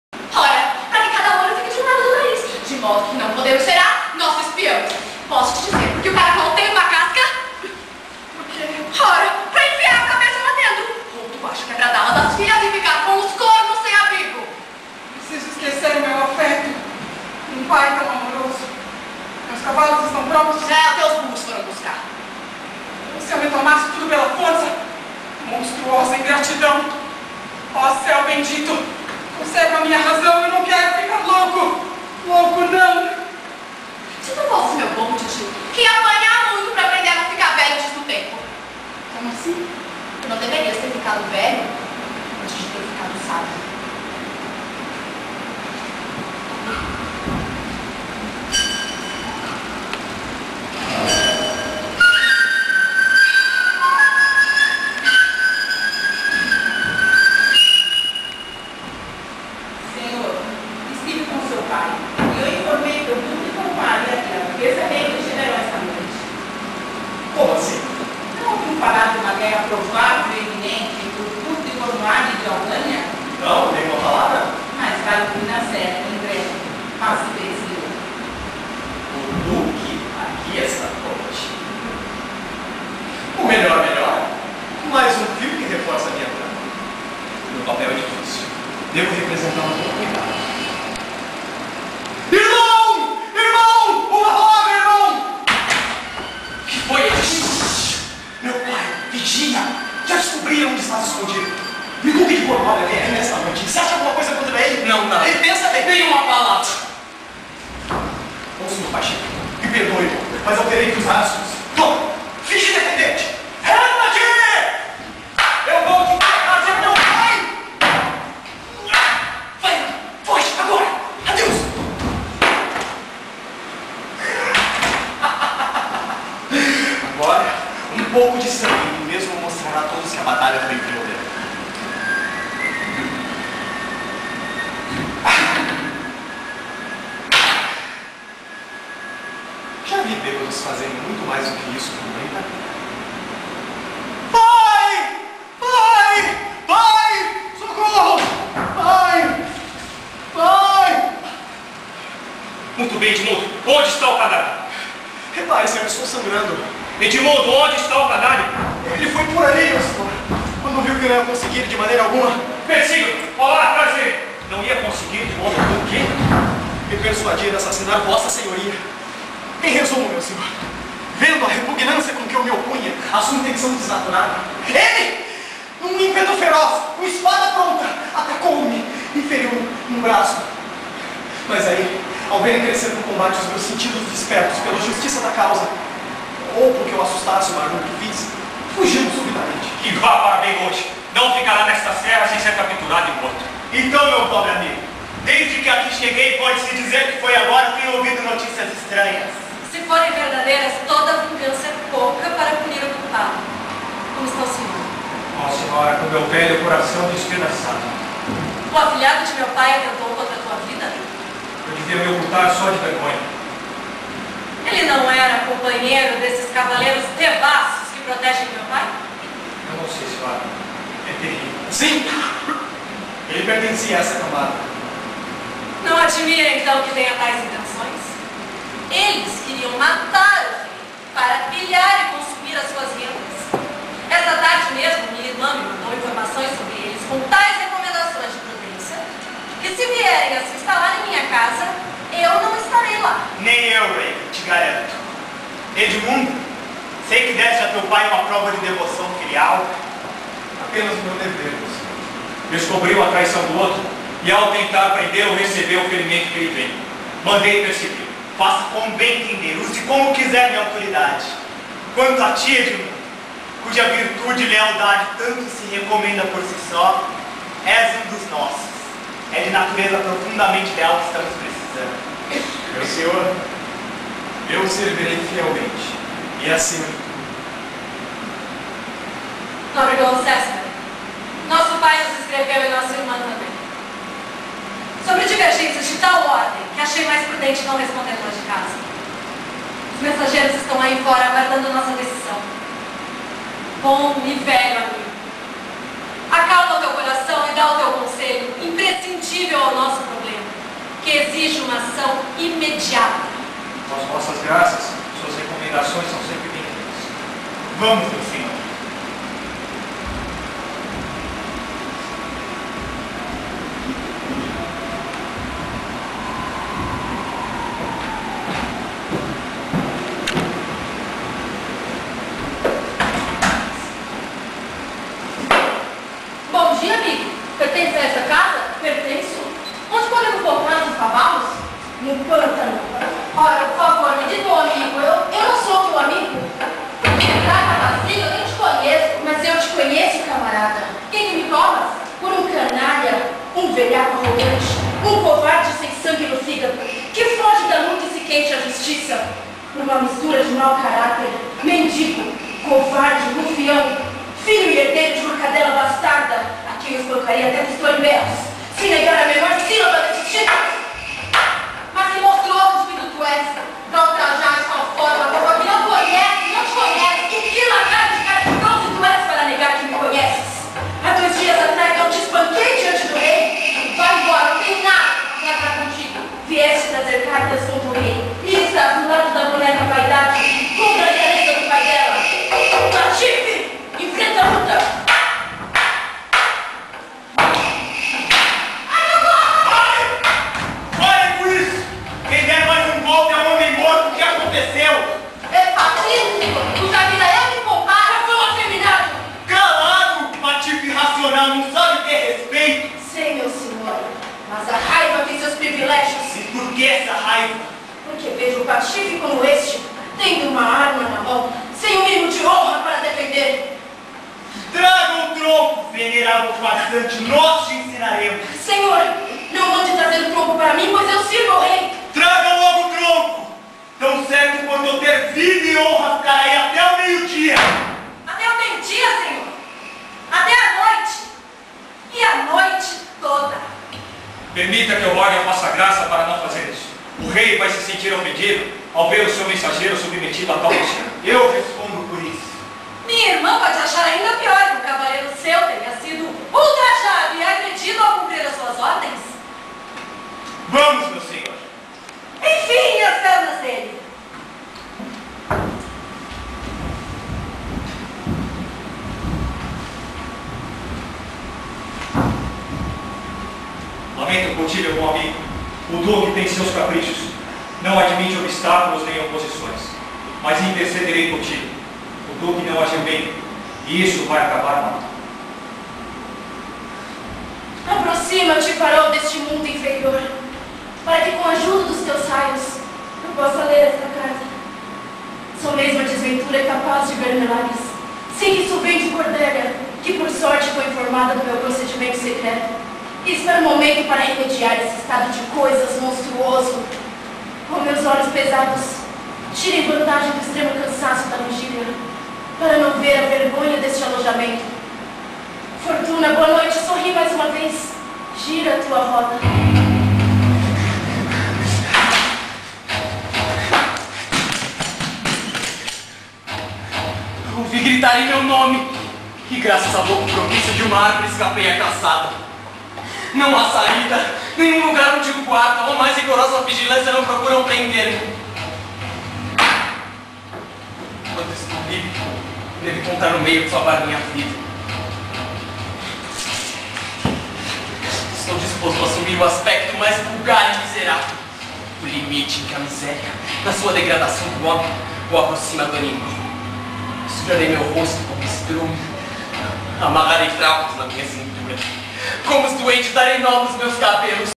PeÃ§a Rei lear parte 4
PeÃ§a apresentada pelo grupo PA4b e Pa5 da Escola de Teatro Macunaima